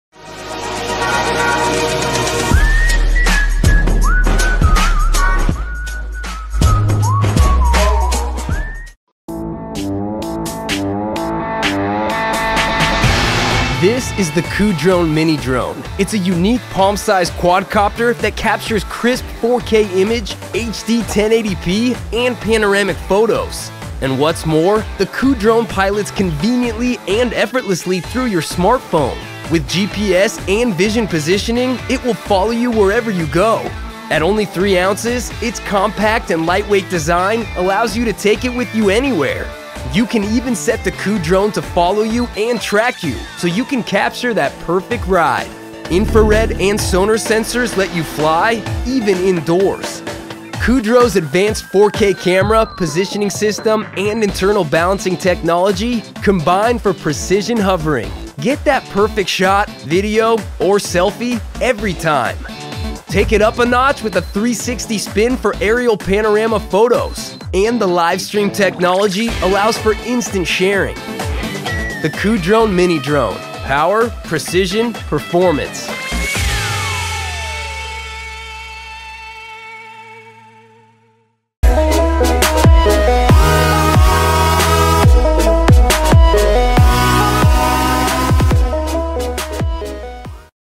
Kudrone: 4K Camera Nano-Drone With GPS Auto-Follow
Palm-Sized Drone With GPS Auto-Follow and 4K Camera With Panoramic Shots For Precision Photography